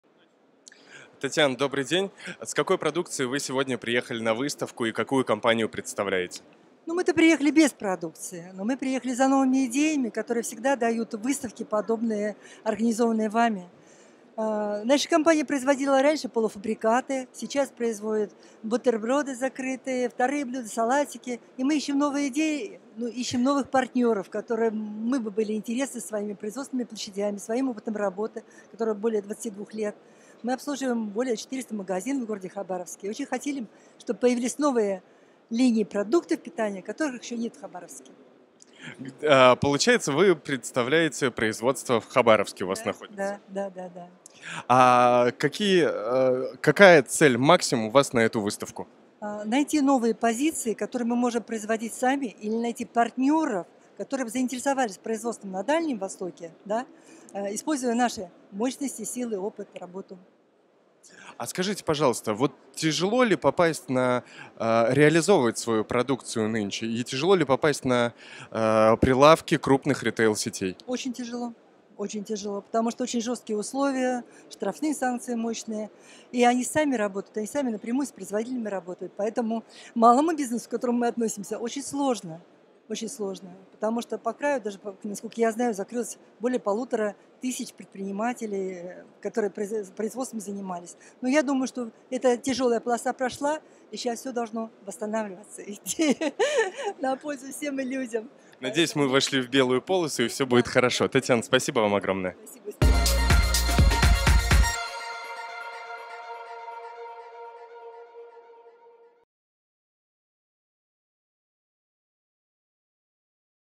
Татьяна Власова, Равиоль
Татьяна Власова, Равиоль, на WorldFoodMoscow2019 приехала за новыми идеями и партнерами.